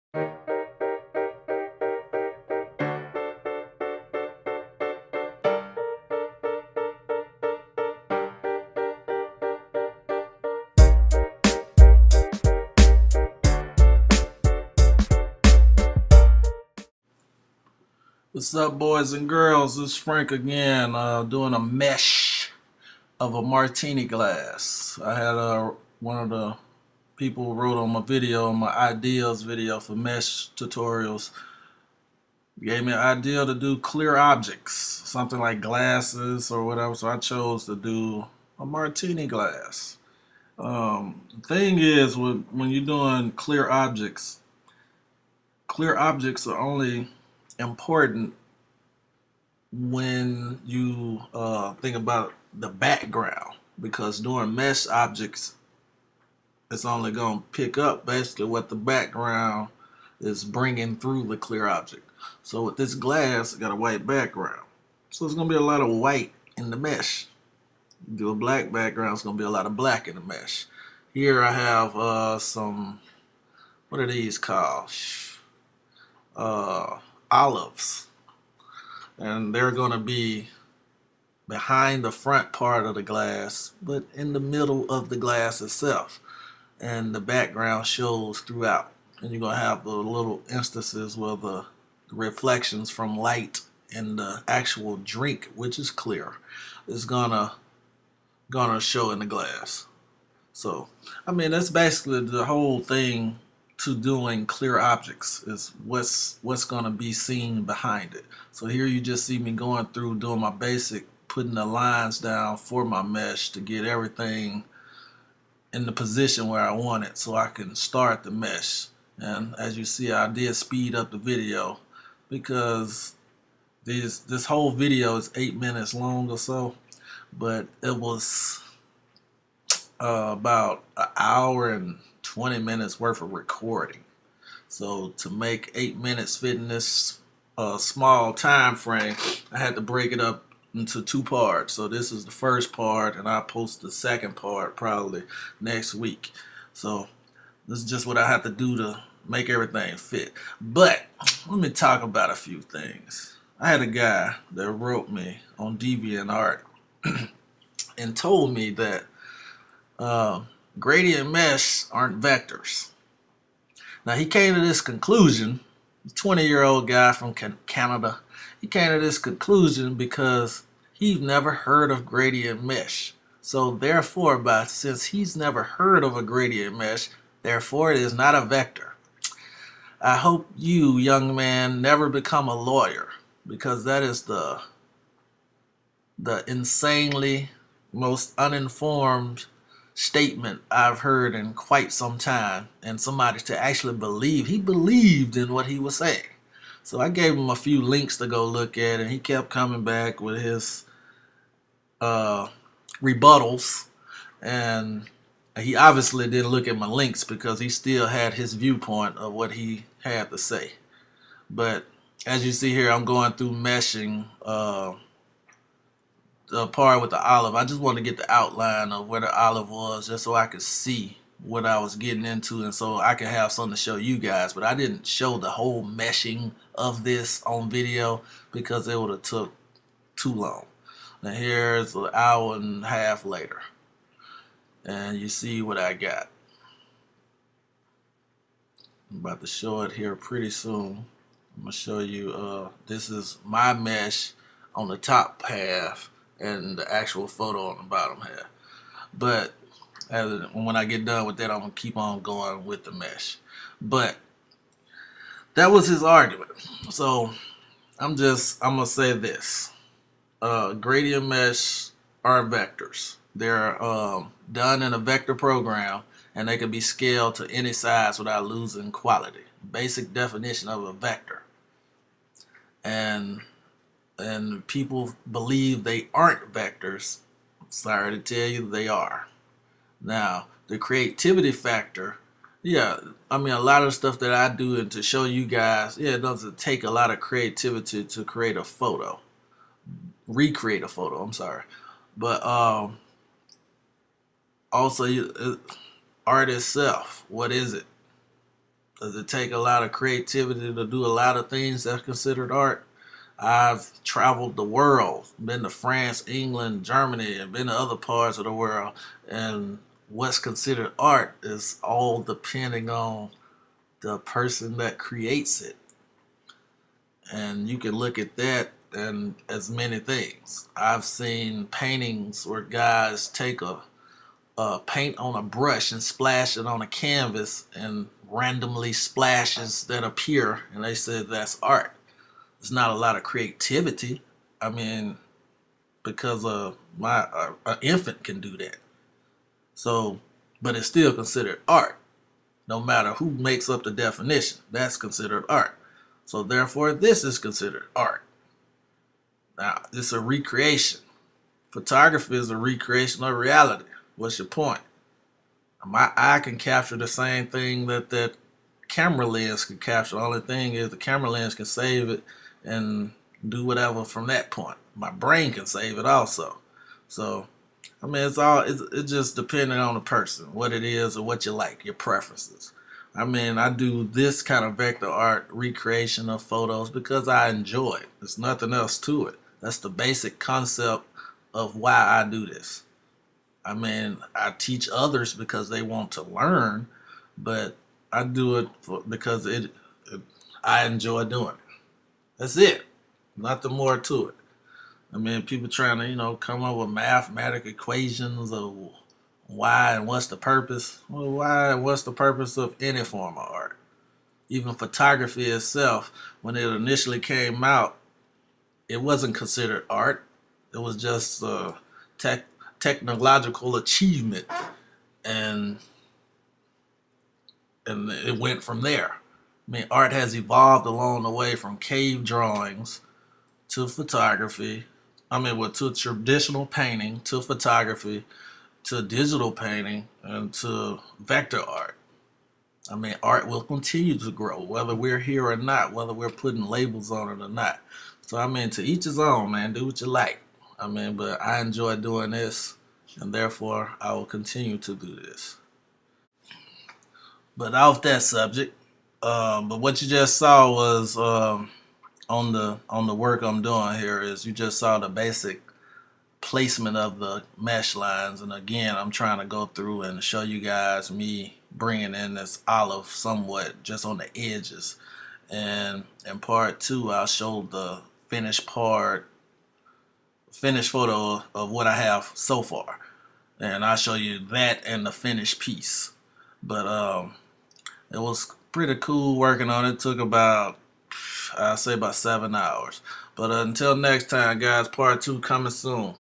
How to Gradient Mesh a Martini Glass Tutorial (pt.1)
Showing my recreation tutorial of a martini glass (clear object) with the gradient mesh tool. Also I'm discussing the basic definition of what is vector.

What is vector? Vector art is the use of geometrical primitives such as points, lines, curves, and shapes or polygon(s), which are all based on mathematical equations, to represent images in computer graphics. Vector art can be scaled to any size and retain its sharpness and detail at no loss in quality. Vectors are most commonly done in programs such as Adobe Illustrator, Macromedia Freehand and CorelDRAW.